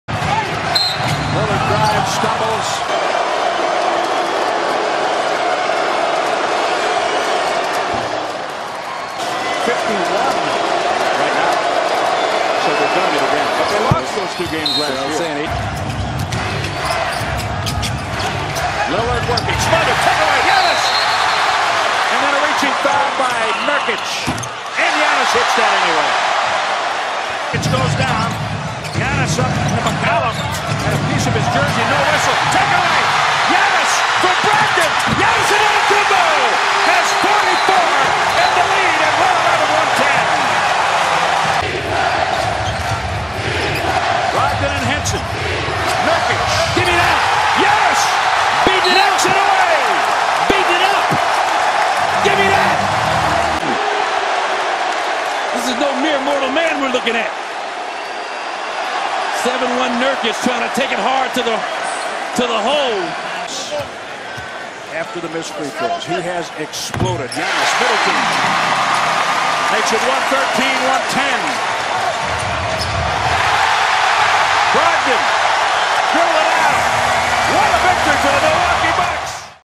Giannis Antetokounmpo CLUTCH MODE! Steals, Scores, then Blocks for the Game Winner
Clutchhh